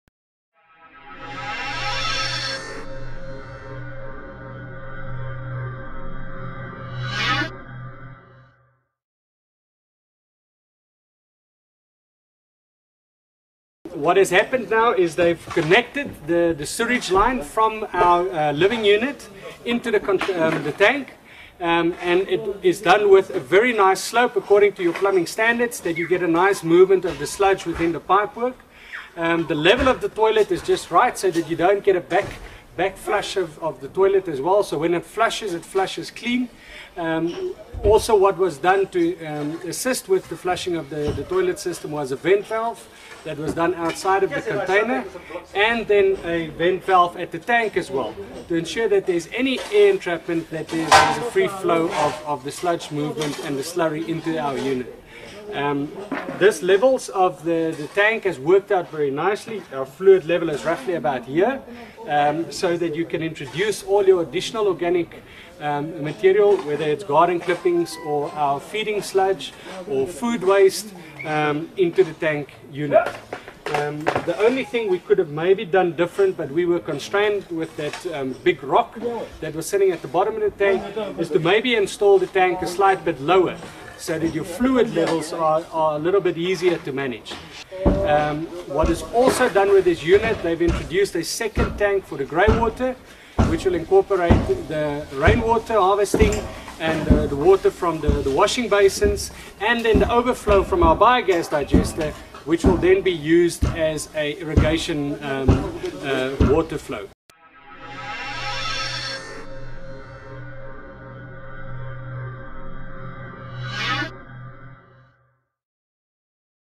Biogas digester Connecting the Plumbing Little Green Monster Wally Weber 1
The Biogas digester is the latest in the Renewable Energy development. Designed to be installed as a retrofit model in a domestic environment to produce methane gas for cooking and heating. Its clean, maintenance free and delivers pressurized biogas. This video shows in detail the correct procedure for connecting the plumbing.